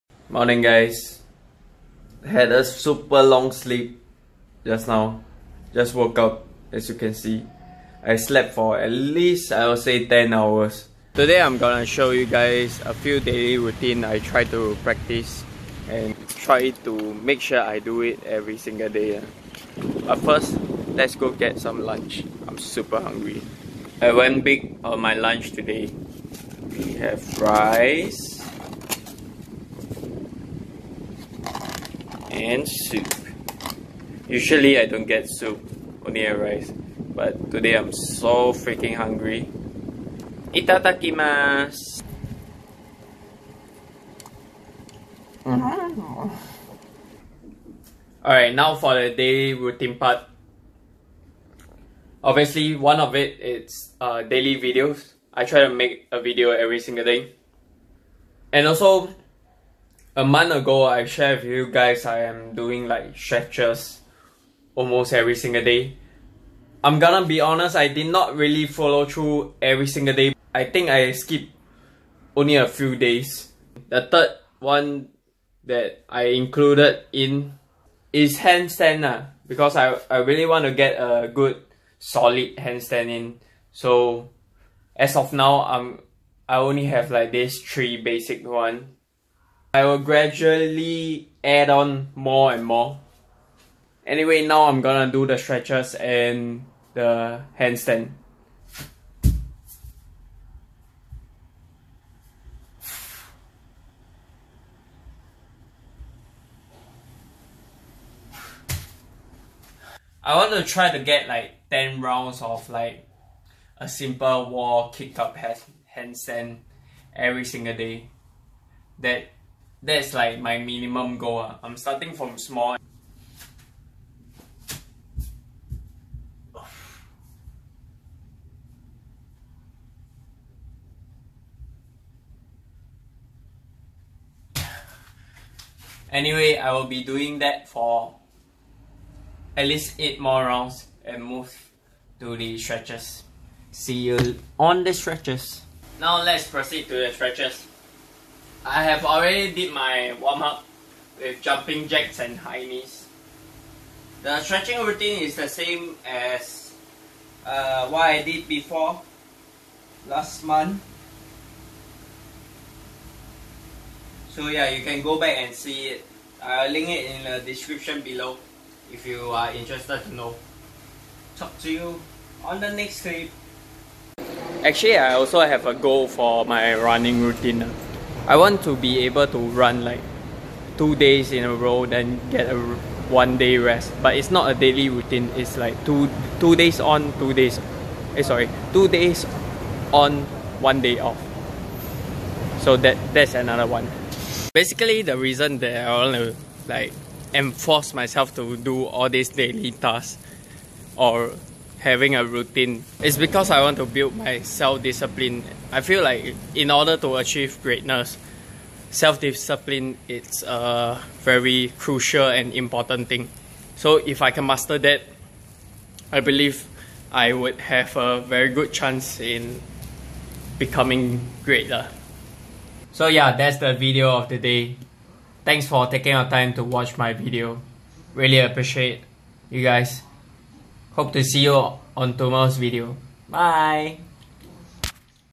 Daily Routine
Daily routine. On this video i talk about my daily routine. Or my daily tasks i give myself to complete.

In order to achieve greatness. I feel it's important to have a daily routines/tasks that bring you closer to greatness.

So this is my little routine for now. I will add more as i go along.